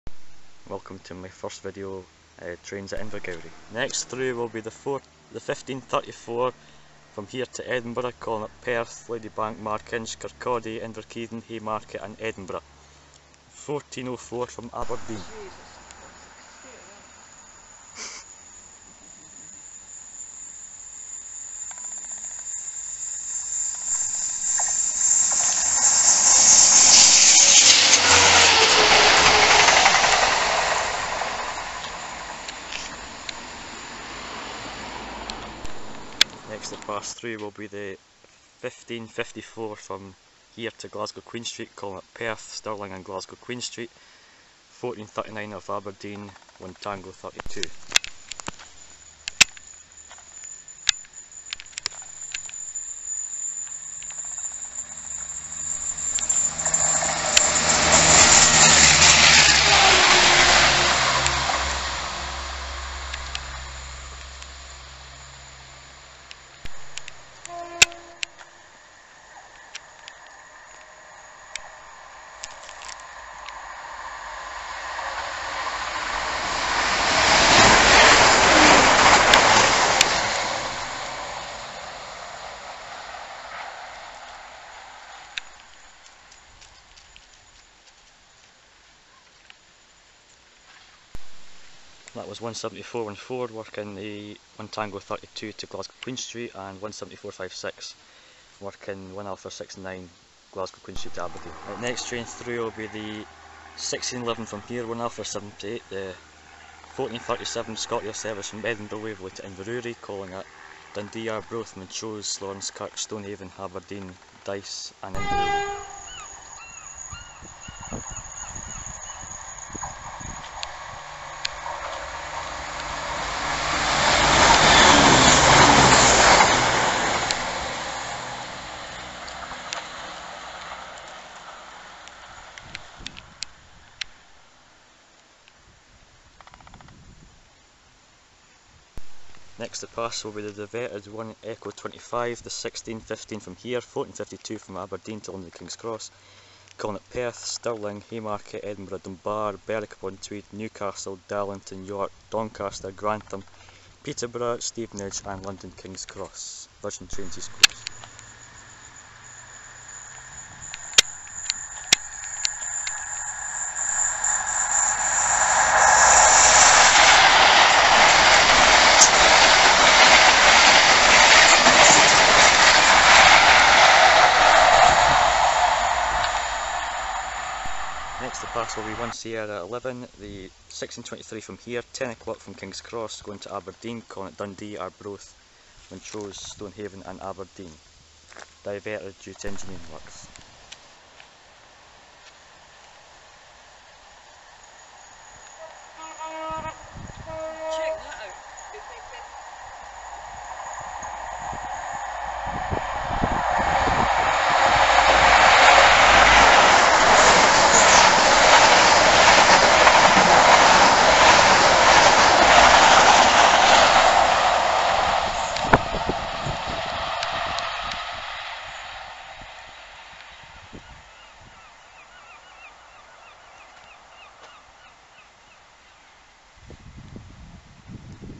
Series 1 Episode 1 | Trains at Invergowrie | Fife Diverts | 10/09/16
Well, my first station video is now live!!
It includes the usual 170s from Aberdeen - Glasgow but also due to engineering works on the Tay Rail Bridge, some diverted Aberdeen - Edinburgh 170s.
A couple of bonuses in this video include two HST's from Aberdeen and London as well as 170414 in 'Borders Railway Livery' going to Glasgow.
Not bad seeing 170414 in my first video!!

I apologise for the poor video quality and sound, this will hopefully be sorted out soon.